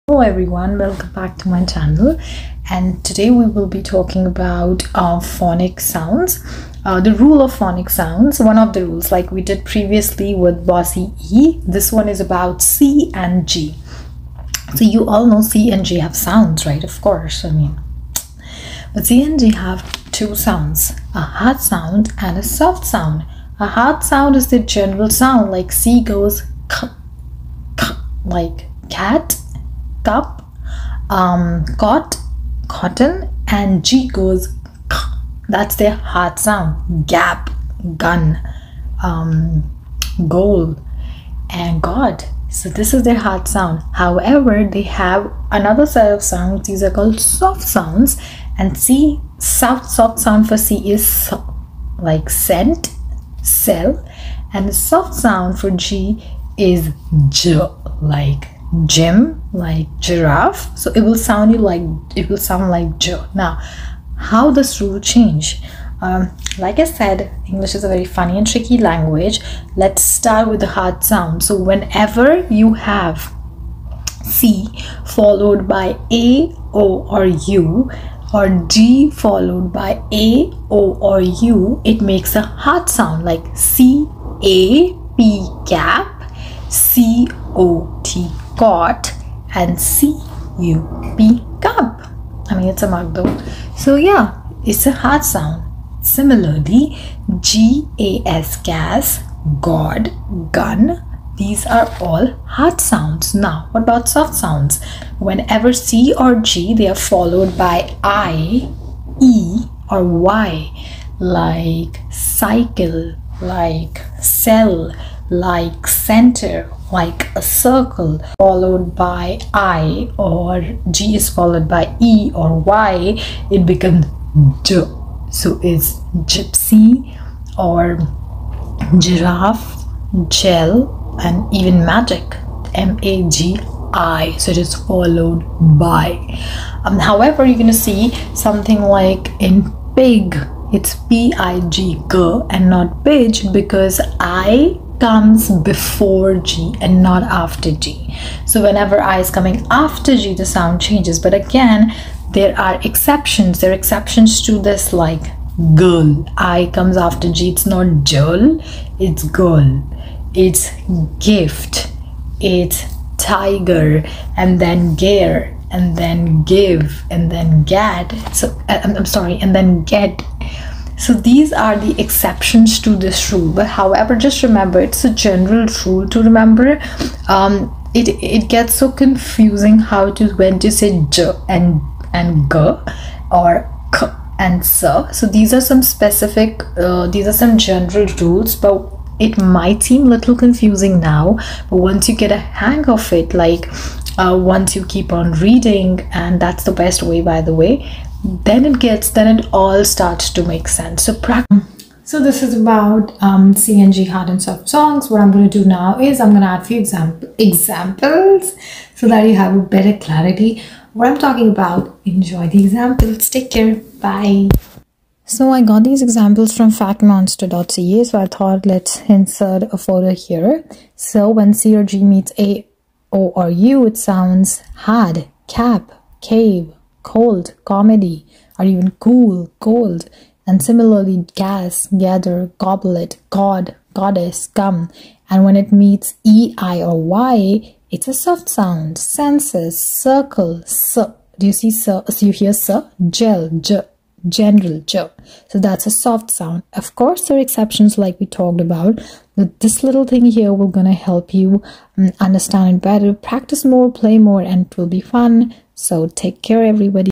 Phonic rules part 2 | Hard and soft C and G sounds
Phonics part 2 - Hard and soft C and G sounds.

C and G sounds ciukd be confusing if one is not aware of hard and soft sounds

C and G have hard sound /k/ or /kuh/ and /g/ orv/guh/ when they are followed by a, o ,u.

Cat
Cot
Cup
Gap
God
Gun

C and G have soft sound  /s/ or /suh/ and /j/ or /juh/ when they are followed by I, e, y

Cell
cite
Giant
Giraffe

There are exceptions as well
Give, Gone, Girl, Tiger etc.